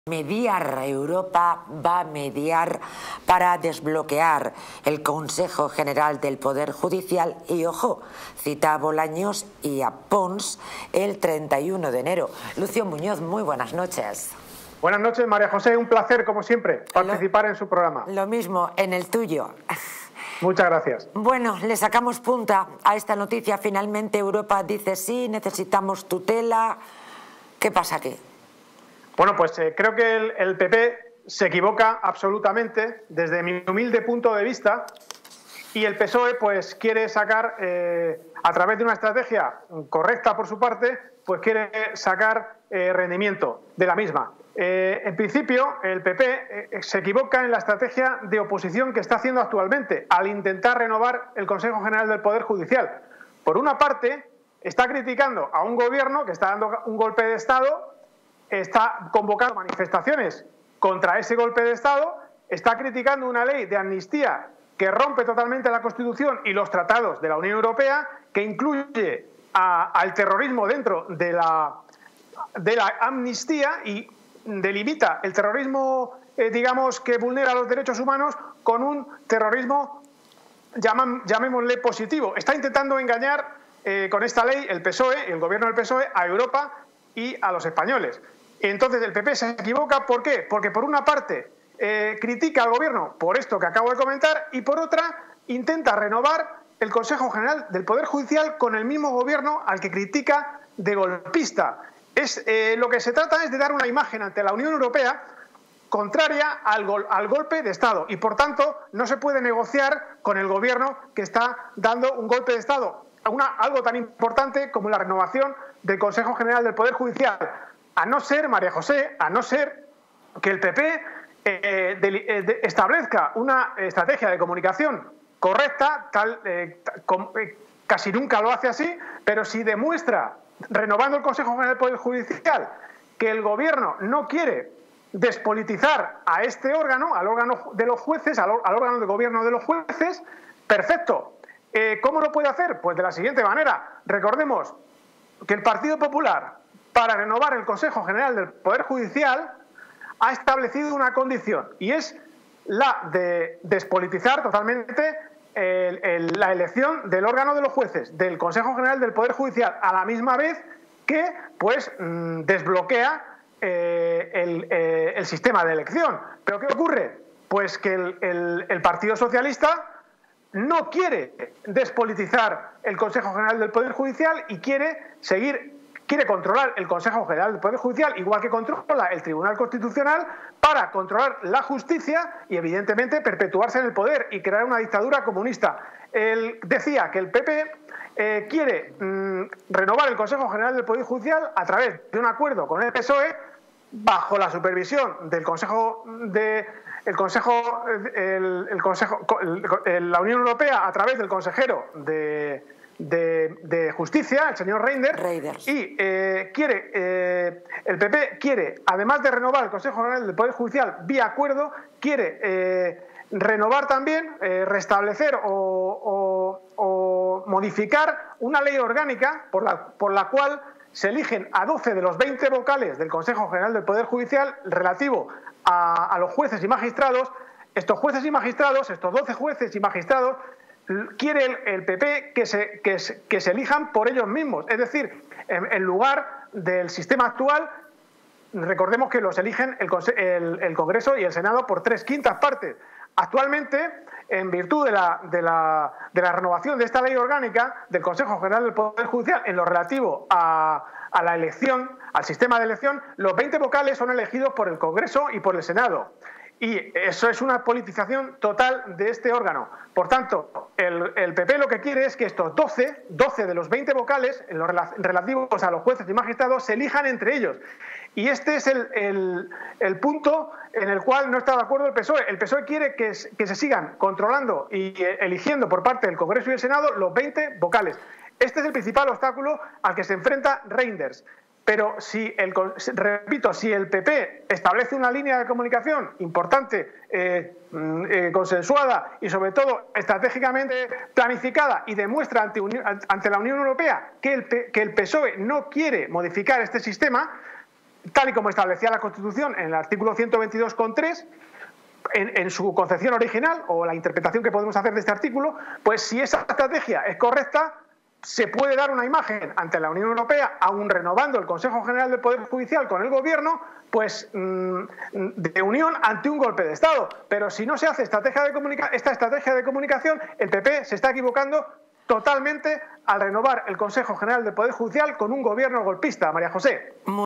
💥ESCANDALOSO💥Reynders acepta mediar para desbloquear el CGPJ y cita a Bolaños y Pons el 31 de enero
¡Escándalo en Europa! Reynders acepta mediar en la crisis del CGPJ y cita a Bolaños y Pons en una reunión el 31 de enero. ¡Entérate de todos los detalles en este vídeo de DistritoTV! Además, te contaremos las últimas noticias sobre Pedro Sánchez, Feijóo y la polémica propuesta de amnistía. ¡No te pierdas este vídeo y mantente informado!

0:00: 🔥 Europa acepta mediar para desbloquear el CGPJ y cita a Bolaños y Pons el 31 de enero. PP y PSOE en estrategias opuestas.
3:04: 🔥 El PP busca despolitizar la elección del Consejo General del Poder Judicial, mientras el PSOE busca mantener control y perpetuarse en el poder.
4:09: 💥 Reynders acepta mediar para desbloquear el CGPJ y cita a Bolaños y Pons el 31 de enero